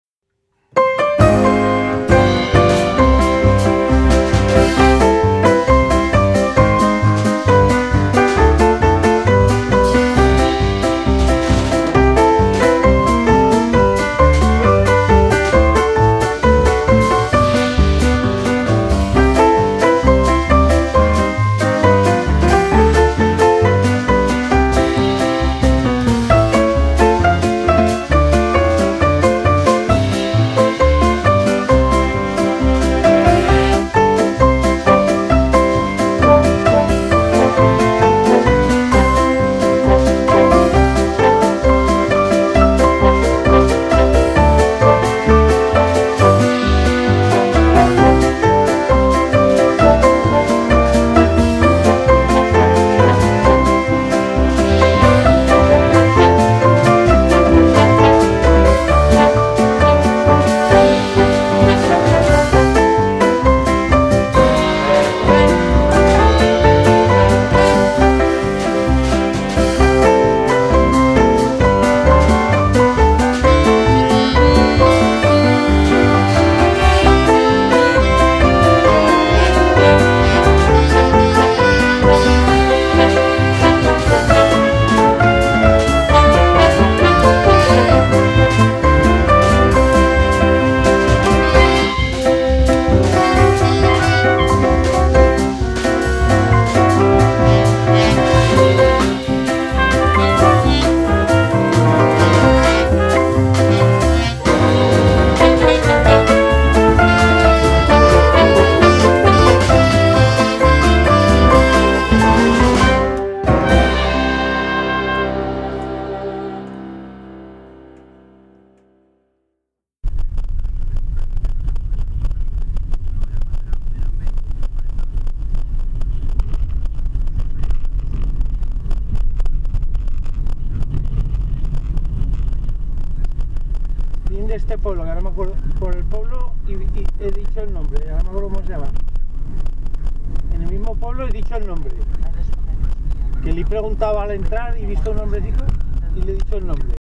Valdastillas 2
Valdastillas es un municipio de España, en la provincia de Cáceres, Comunidad Autónoma de Extremadura.
Está ubicado en las estribaciones de la Sierra de Tormantos, en la comarca del Valle del Jerte.